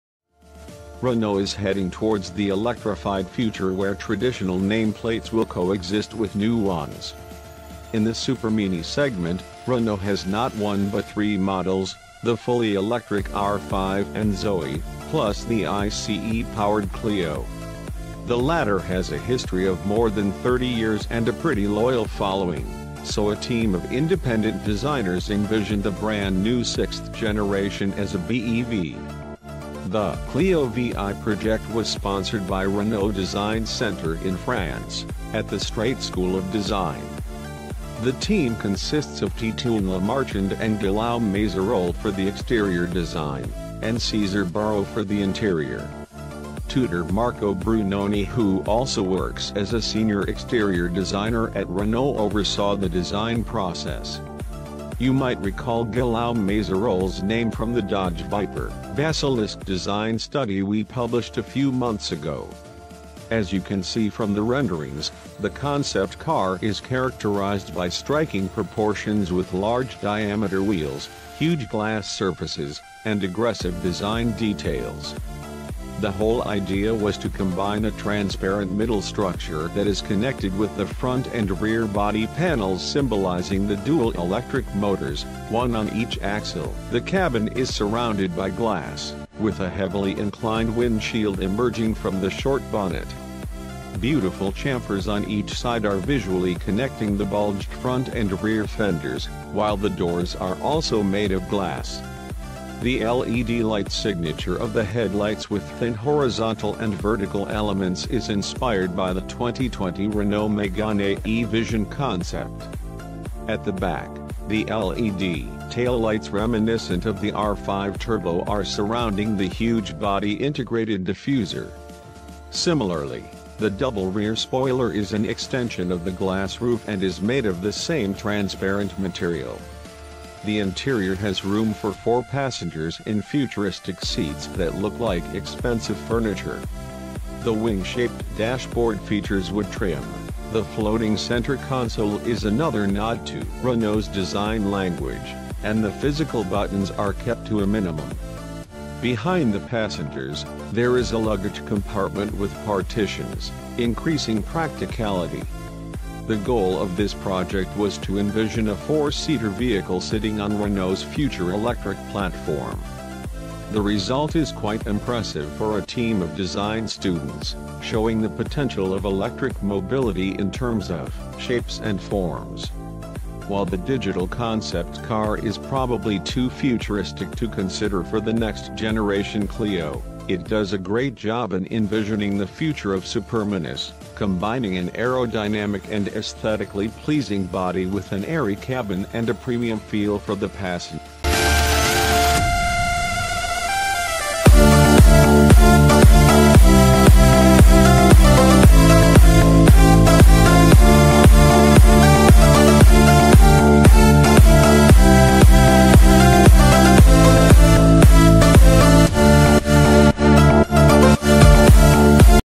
Future Renault Clio EV Envisioned By Design Students
Renault is heading towards the electrified future where traditional nameplates will coexist with new ones. In the supermini segment, Renault has not one but three models: the fully electric R5 and Zoe, plus the ICE-powered Clio. The latter has a history of more than 30 years and a pretty loyal following, so a team of independent designers envisioned a brand new sixth-generation as a BEV.

( Images are used only for representation purposes )